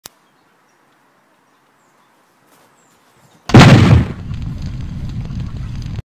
Bang - Backyard Antics Needed Grenade
We found an old strange grenade in the backyard and decided to see if it would actually work.